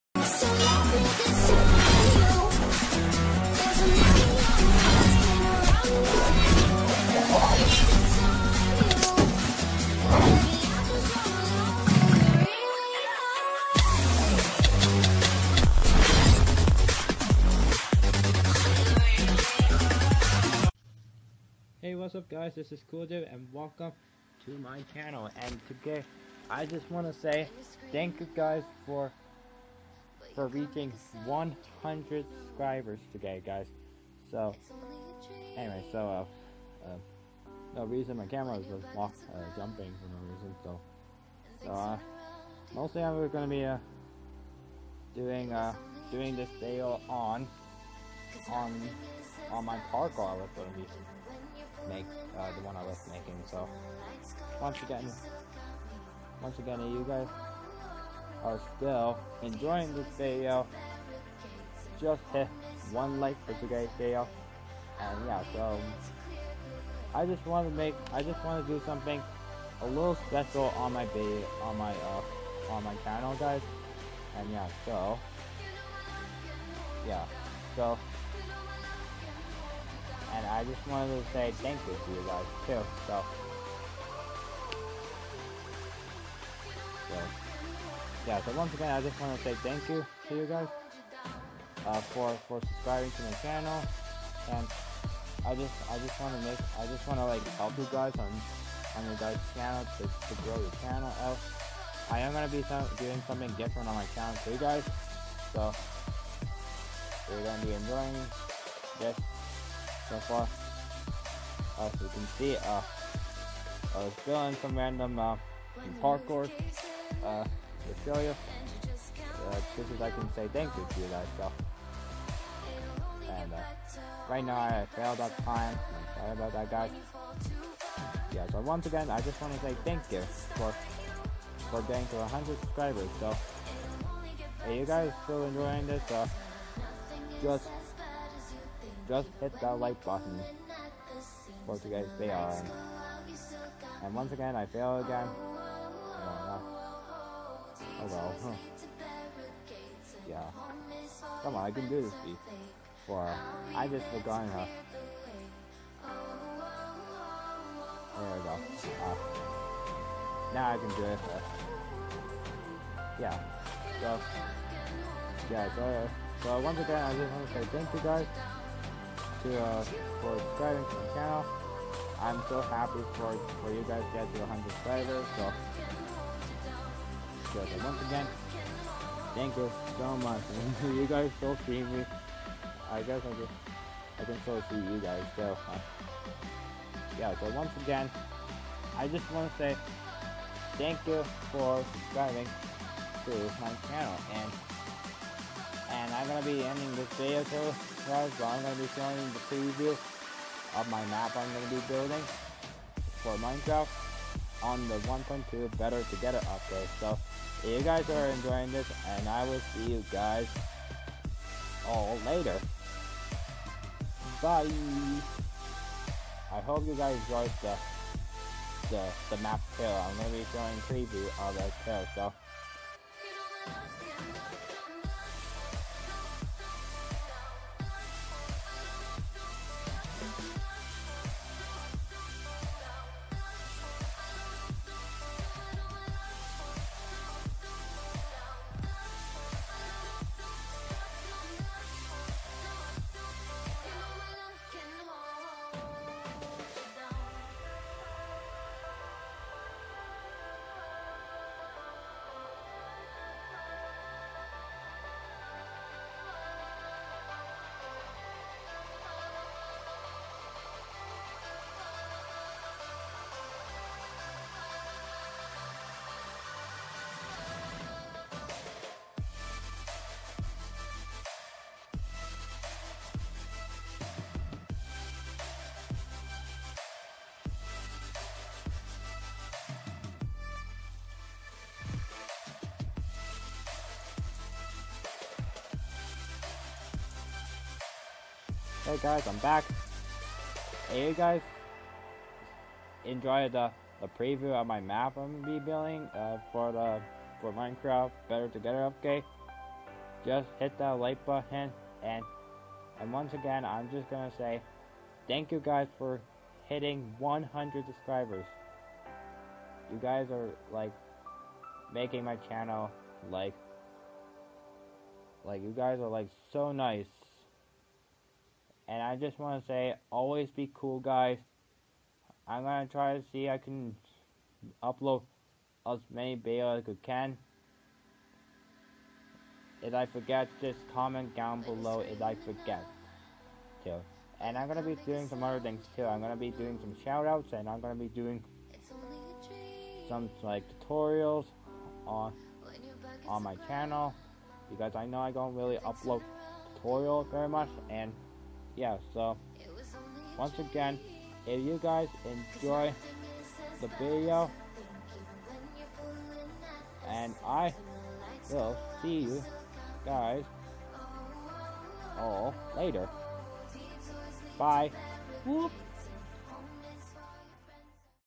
100 SUBSCRIBERS SPECIAL (Thank You Guys (Parkour Map Preview)
Hey guys I just want to say thank you guys for getting 100 subscribers on my channel you guys are very nice and I want to help on your guys channel so I am gonna do a shoutout on my channel and do more tutorial on my personal and yeah guys so thx you guys for watching the video I hope you guys enjoy it 😊